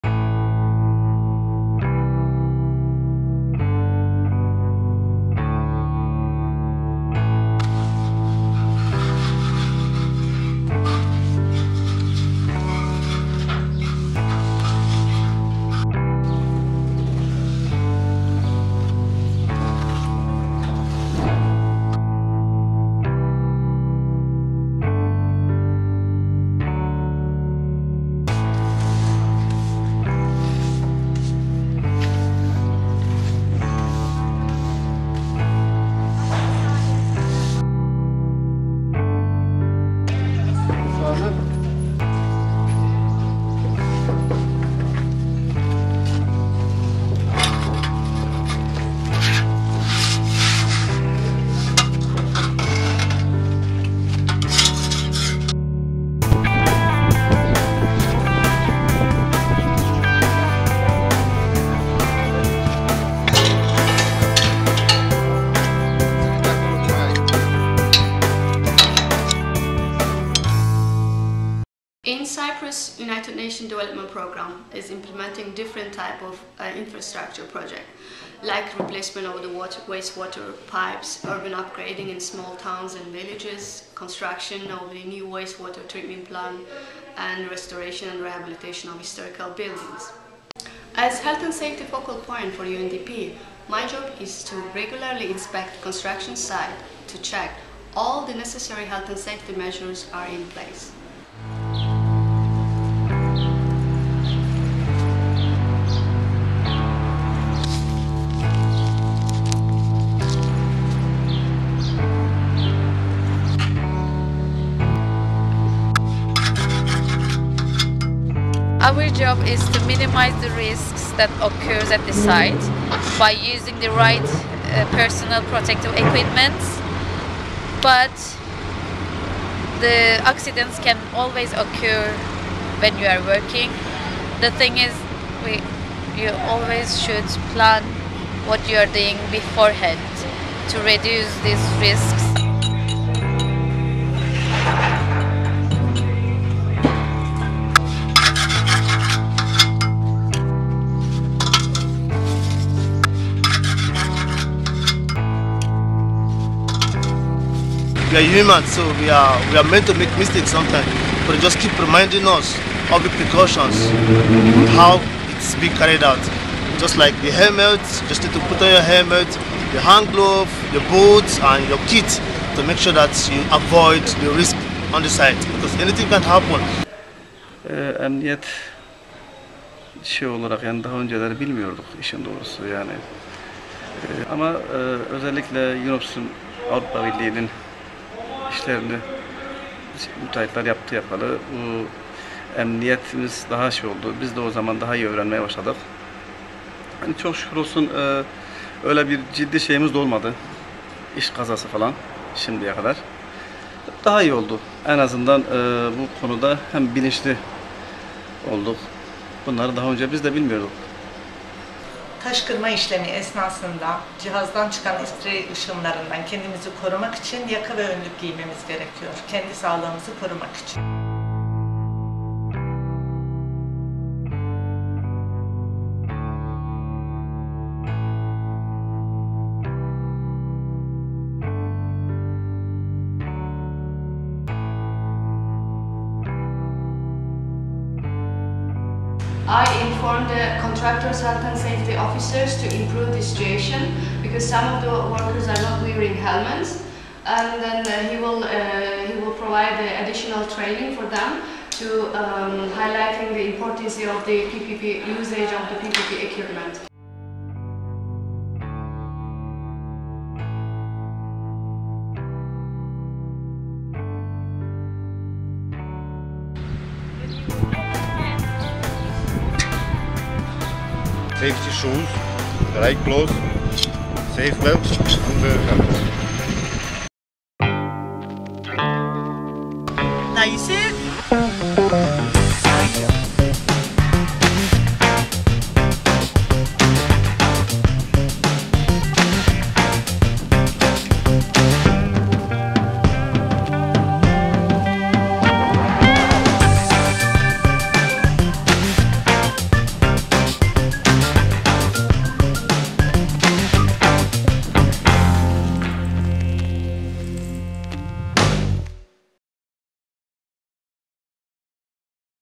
28 April - World Day for Safety and Health at Work
With this video UNDP-PFF Team in Cyprus, the European Union, its contractors and workers mark 2012 World Day for Safety and Health at Work (28 April). With more than 55 civil works contracts awarded in the past five years -- ranging from 10,000 to 25mln Euros each -- and approximately 800 workers employed, health and safety is a priority issue for UNDP in Cyprus. From coffee shops restoration sites in small villages, the almost completed  market site in old Nicosia, improvement and rehabilitation of minor and major town centres, the construction site of the new Nicosia waste water treatment plant, to the renewal of water distribution networks, UNDP Partnership for the Future in Cyprus  regularly checks that health and safety regulations are observed thoroughly on-sites and this is done by  constant liaison with the sites' supervisors and contractors as well. On each site visit we spoke to workers, engineers and constructors about the health and safety measures put in place in their work place.